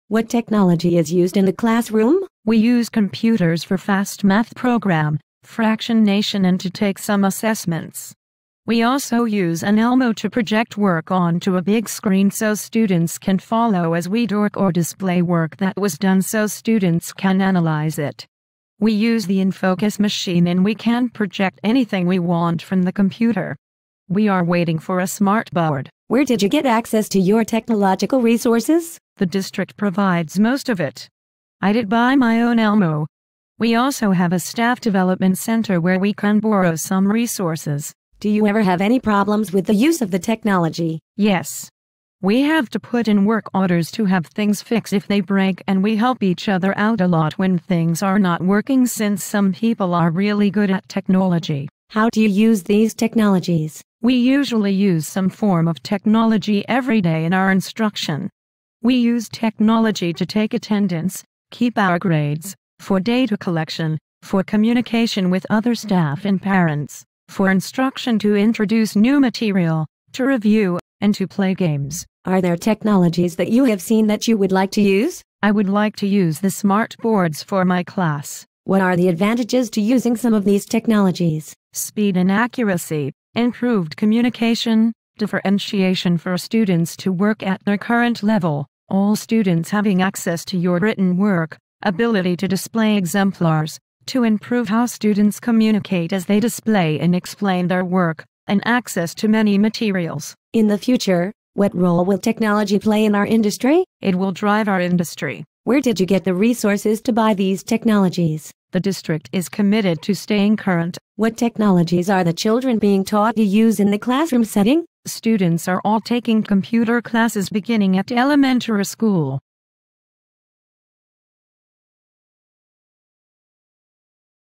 Technology in the Field Interview
I interviewed my 7th and 8th grade Special Education homeroom teacher, Mrs. Shari Stahl, from Hoover Middle School in Kenmore, New York about the technology she uses in her classroom.